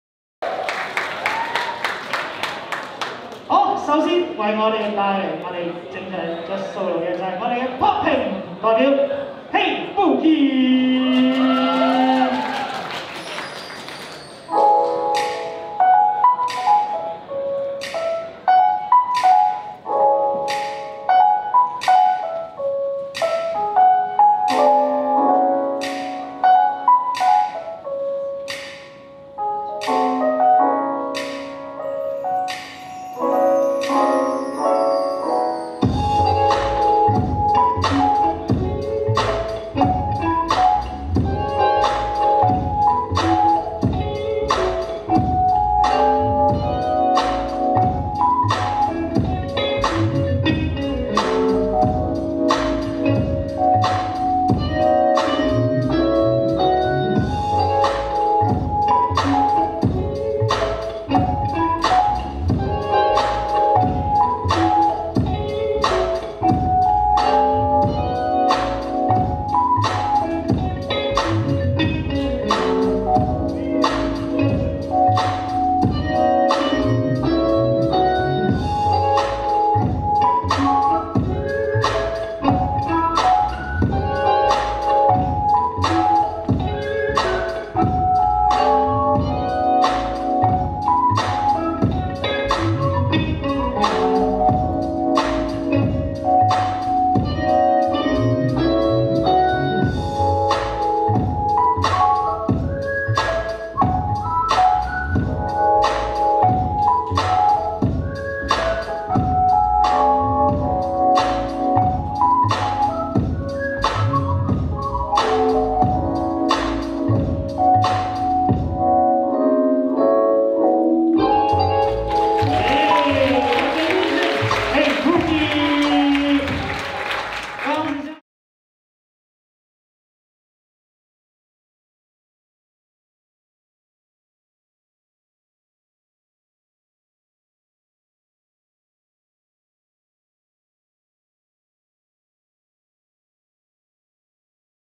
Hay Boogie | Judge Showcase | Popping Battle 1on1 | Hong Kong Street Dance Championships 2020
香港街舞公開賽2020 Hong Kong Street Dance Championships 2020

主辦Presented by：
香港街舞發展聯盟 Hong Kong Street Dance Development Alliance

資助 Supported by: 香港藝術發展局 Hong Kong Arts Development Council
協辦Cooperated with: 鐵仕製作 TS Crew
錄影剪輯 Video Production： Solotop

HKSDDA Instagram： @hksdda_official